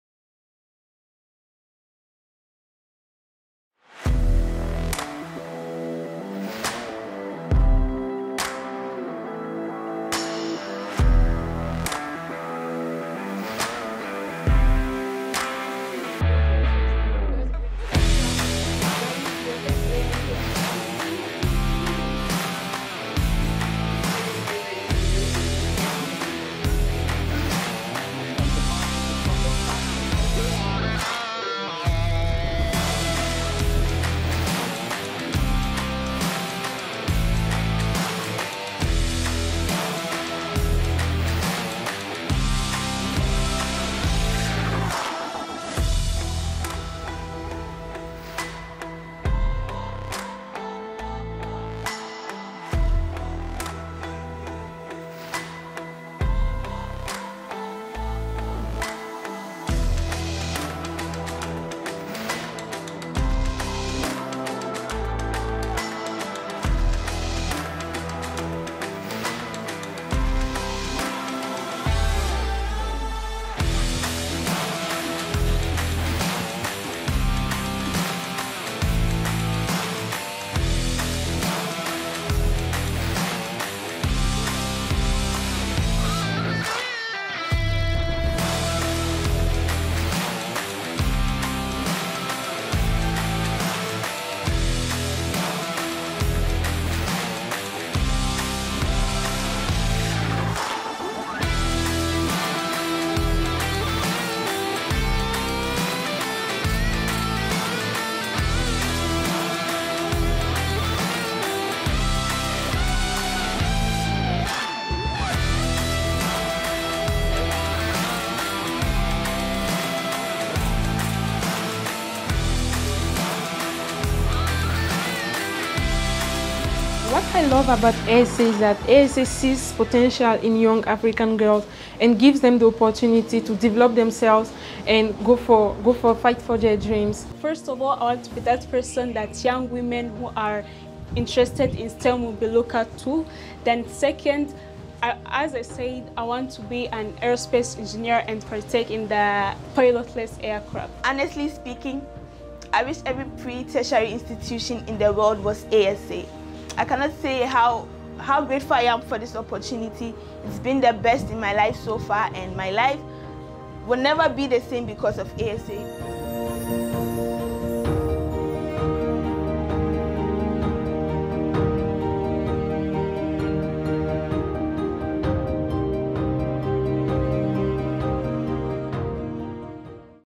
African Science Academy Campus Tour
Every year, we welcome gifted young girls from Africa to study what they are passionate about, Science Technology Engineering and Math (STEM).

At African Science Academy, our approach to learning encourages critical thinking and curiosity. Have a look at what a day in A.S.A looks like...

Admissions application for the 2023/2024 academic year is ongoing!